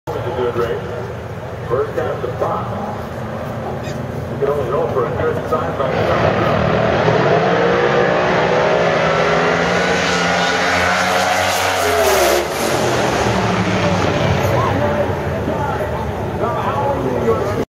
First race I saw from the balcony at the end of the 1/4
You get what 230 mph feels like if you ever saw it on the streets for some reason?